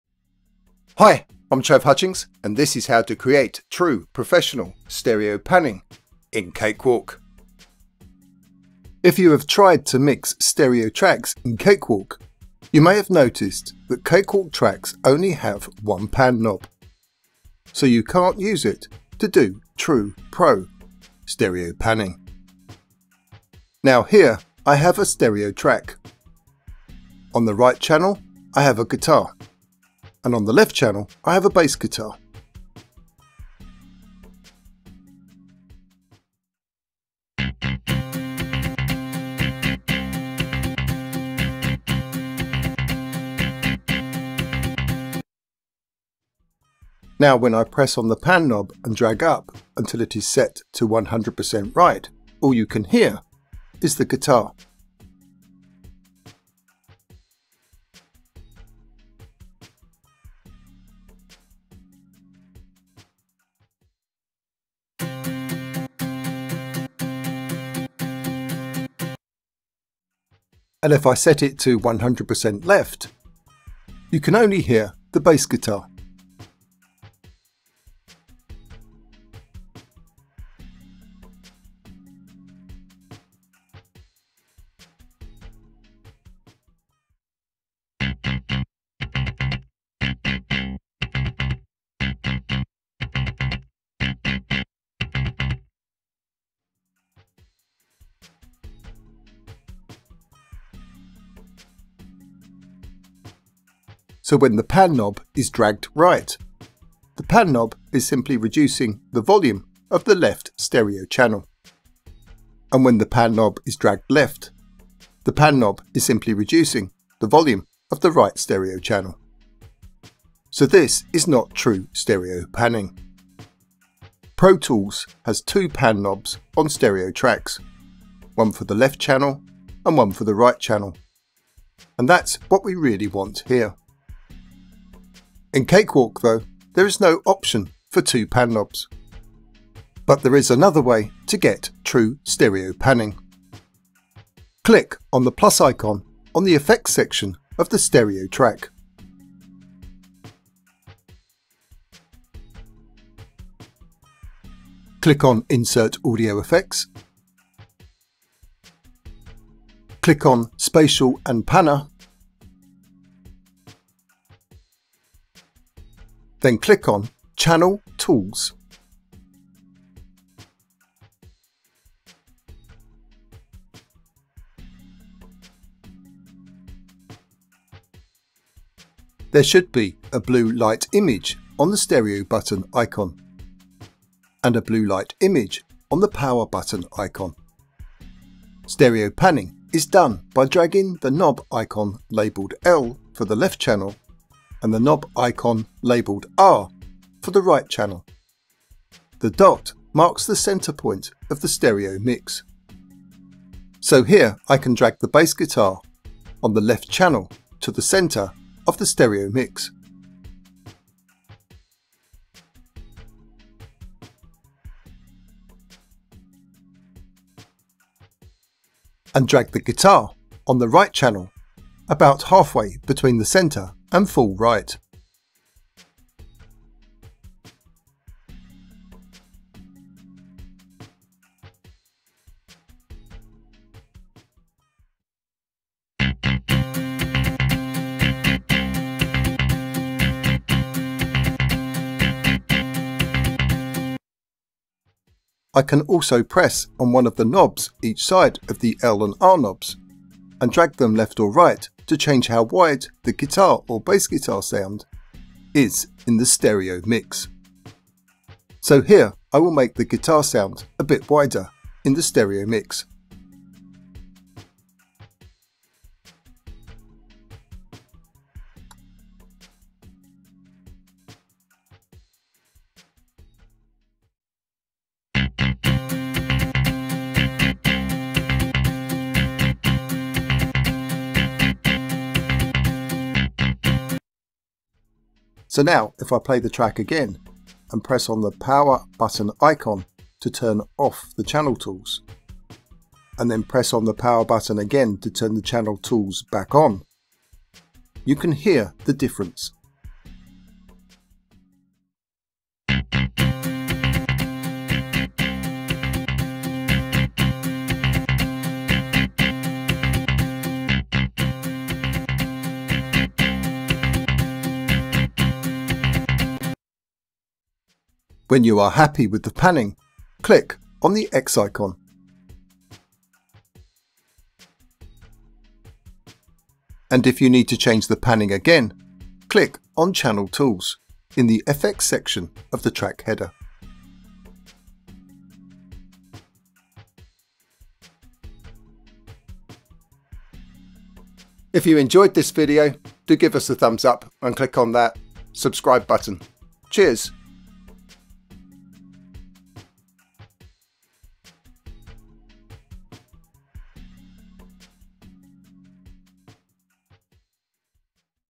True Pro Stereo Panning
How to Create True Pro Stereo Panning in Cakewalk (by Bandlab).